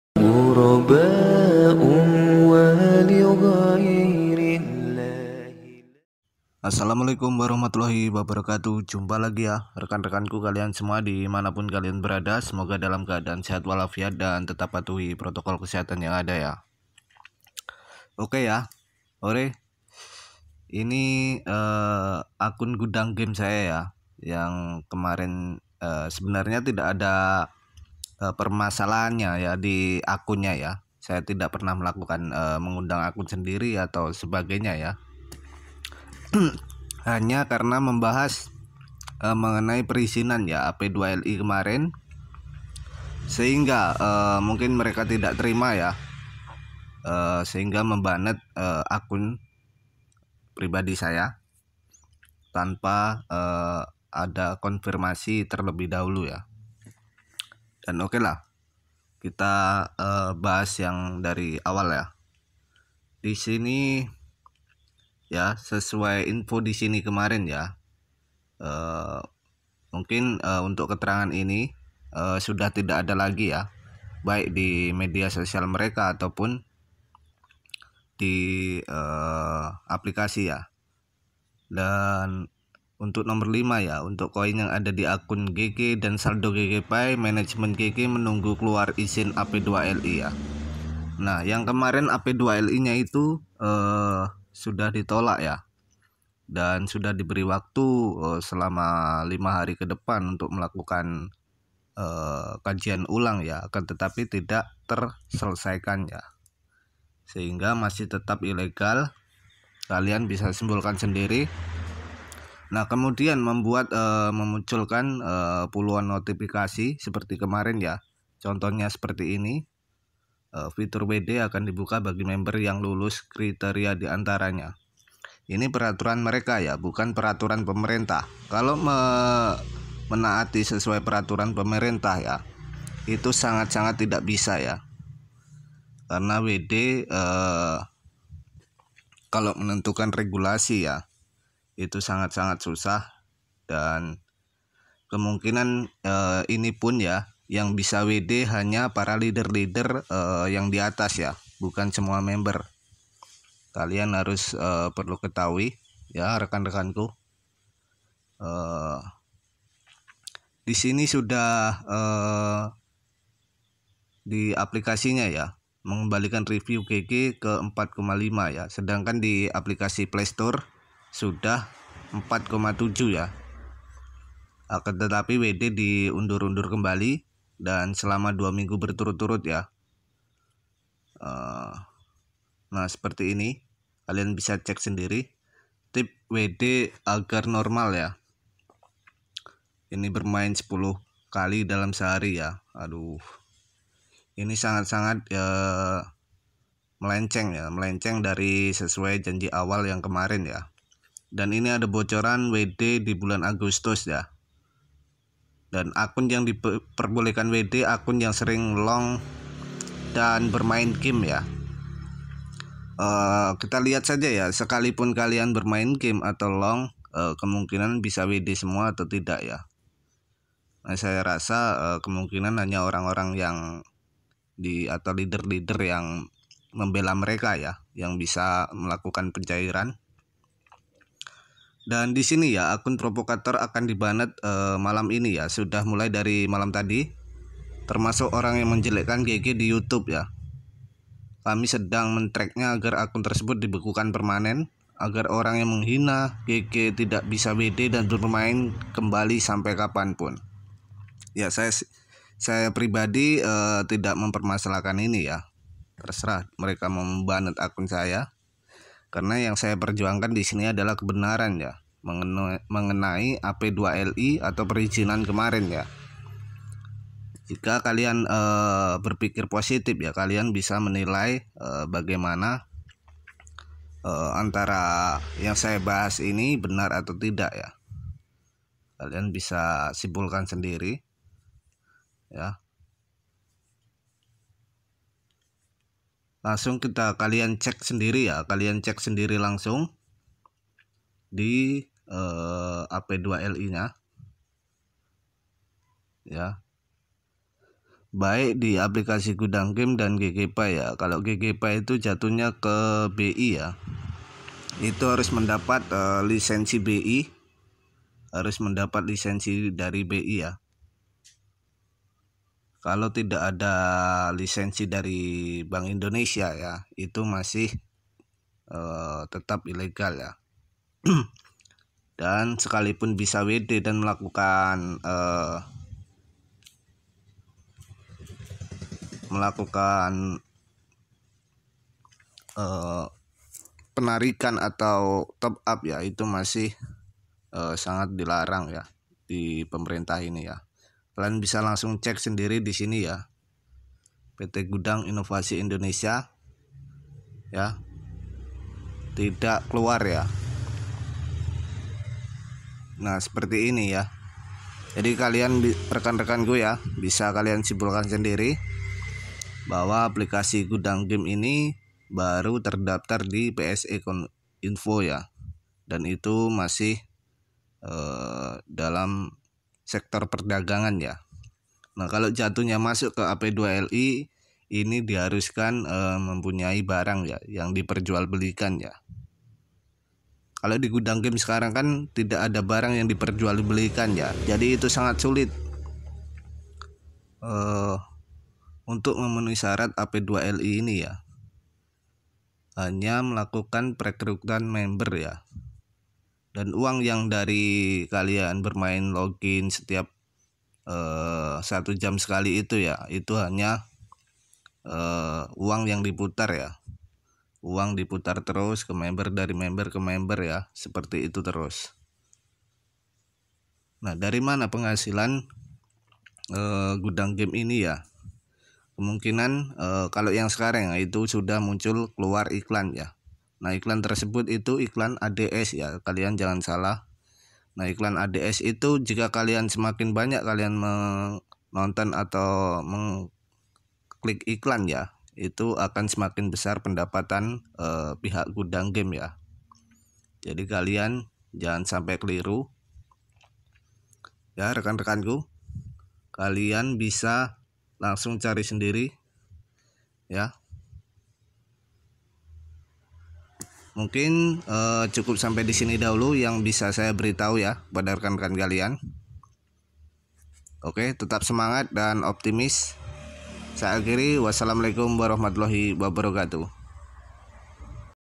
🔴 GUDANG GAME & GGPAY SEKALIPUN BISA WD MASIH TETAP ILEGAL DI PEMERINTAH !!!
Saya ucapkan beribu terima kasih kepada pihak gudang game karena telah mem-banned accaunt gg saya tanpa sebab yang jelas dan tidak ada konfirmasi sebelumnya baik komunikasi chat maupun via telp, dari awal saya pribadi tidak ada unsur menjelekkan dsb, hanya membahas sesuai fakta dan seiring waktu berjalan, bukankah lebih baik tuntaskan dahulu semua legalitas gg, sebelum ramai memunculkan notif bisa kembali wd dengan syarat tertentu dari perusahaan, bukan dari pemerintah, ini negara besar harus ada izin dan aturannya, kalian boleh berwenang di aplikasi tapi ingat masih ada yang lebih berwenang di negara 🇮🇩 ini 🙏

Bahkan sudah jauh hari saya memang berniat dan meminta agar akun gg saya di blok, cek disini karena saya pribadi lebih utamakan masyrakat di luar sana, agar tidak ada yang di tutupi sehingga hanya akan membuat pembohongan ke publik, itu justru lebih membuat malu dan mencoreng nama baik saya terhadap negara yang kita cintai ini

(anda sehat, anda paham)

Agar kalian lebih jelas, silahkan hubungi disini